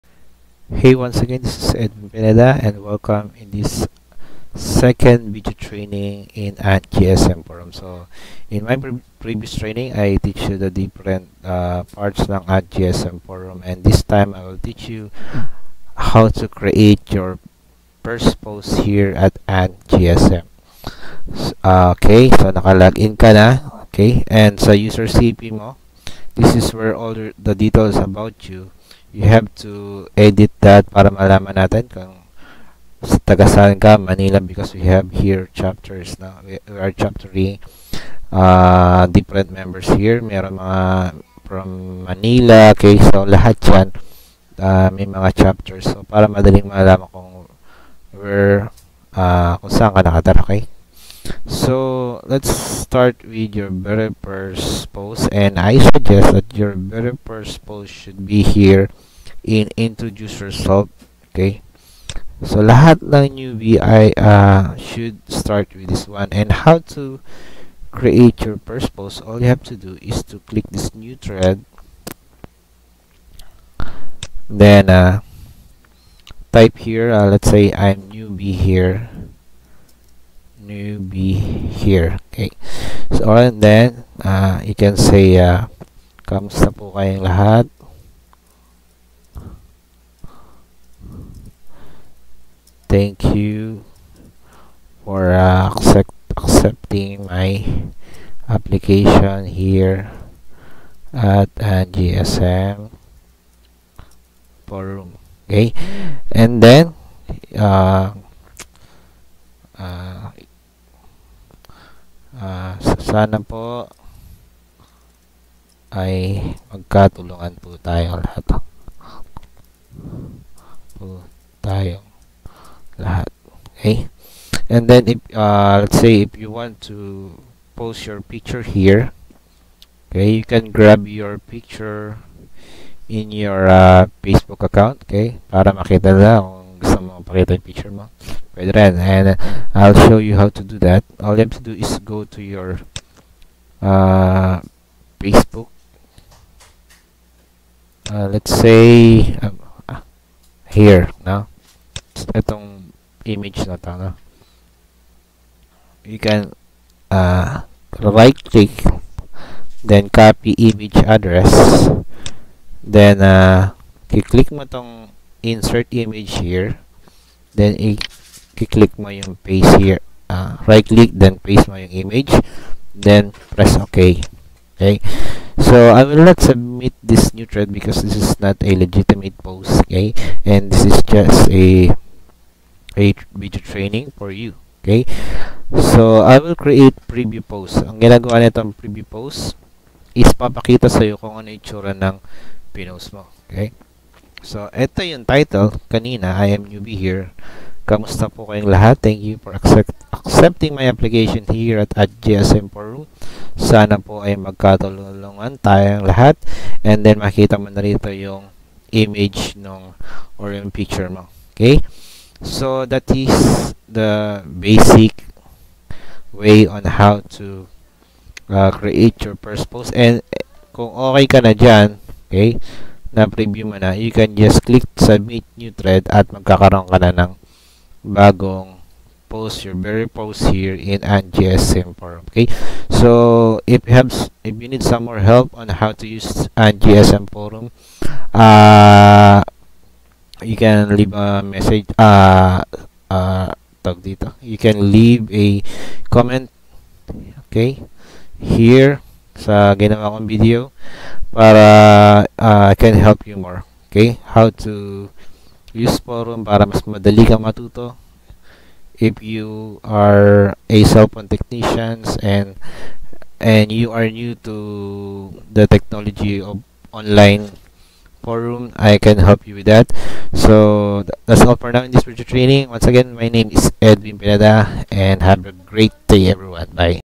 How to create your first post in AntGsm Forum
In this video training I will teach you how to create your first post here in AntGsm Forum.